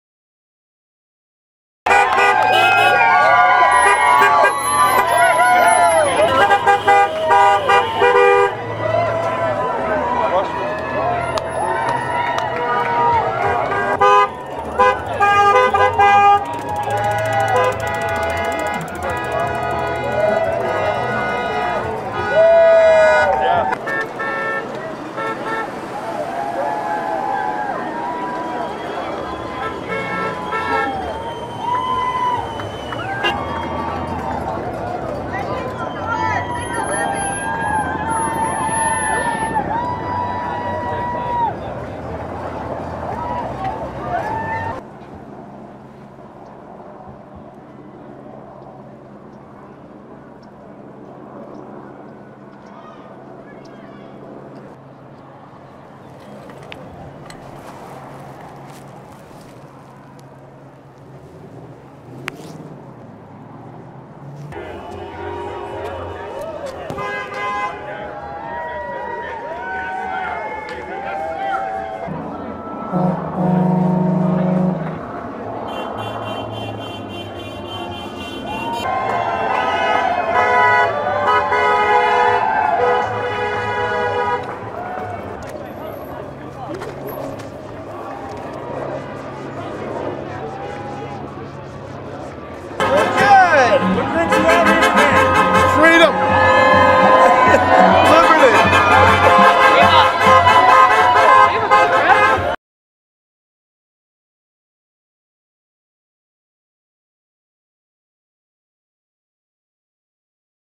Protest outside MN Governor residence against stay-at-home order
For a second day in a row, protesters gather outside the governor’s mansion in St. Paul, Minnesota, urging Governor Tim Walz to end the state’s stay-at-home order.

TWITTER/@GOVTIMWALZ / CBS / AFP